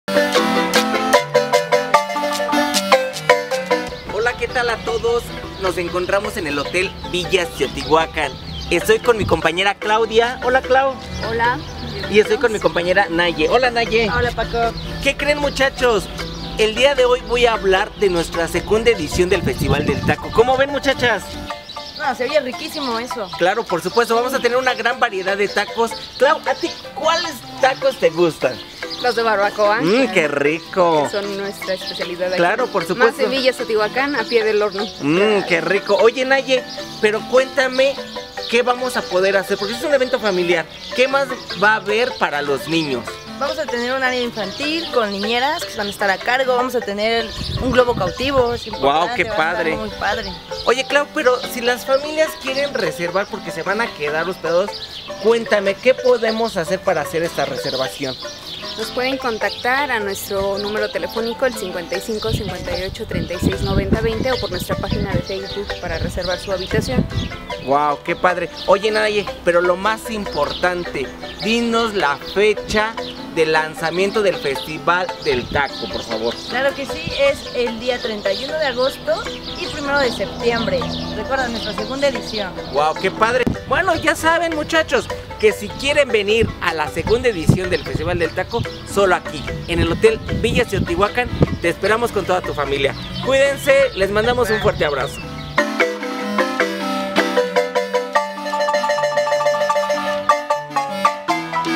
FERIA DE TACO EN VILLAS TEOTIHUCAN
UNA FERIA PARA DEGUSTAR EXCELENTES VARIEDADES DE TACOS
VAMOS A VILLAS TEOTIHUCAN EL 31 DE AGOSTO 1 DE SEPTIEMBREDEL 2019 TE ESPERAMOS.